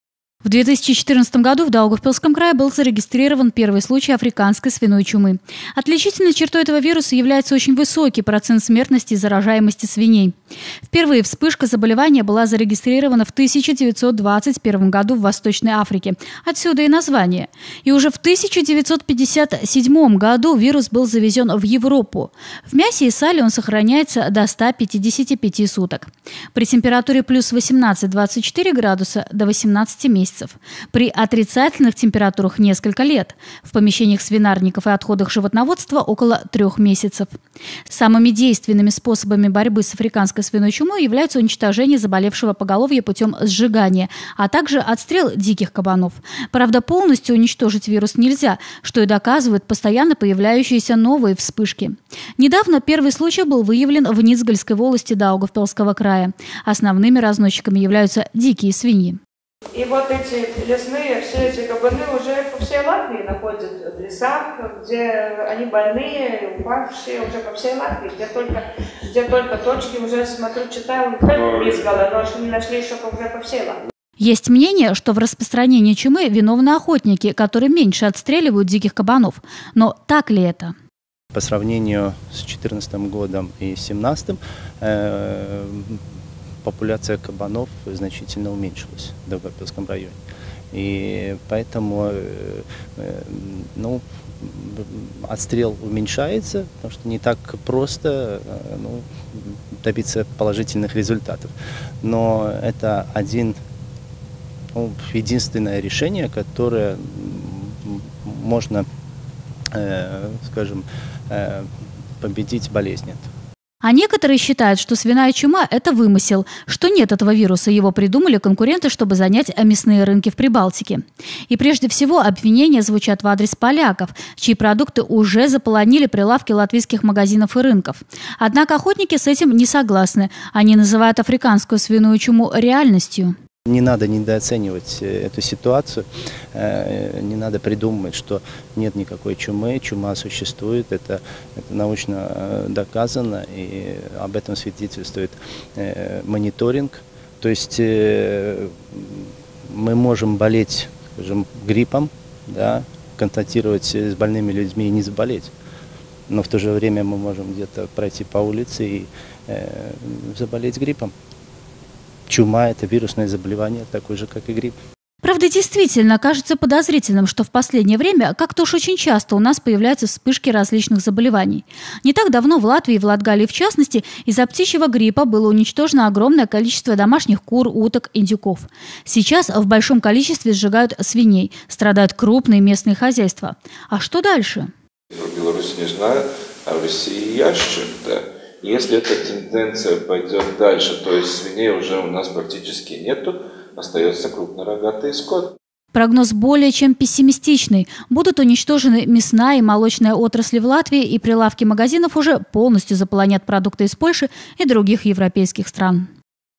В Даугавпилсский край вернулась чума
В последнее время в Даугавпилсском крае все чаще стали обнаруживать больных африканской чумой домашних свиней и диких кабанов. Откуда приходят к нам больные животные и как предотвратить распространение эпидемии? Тему продолжит Елена Костыгова.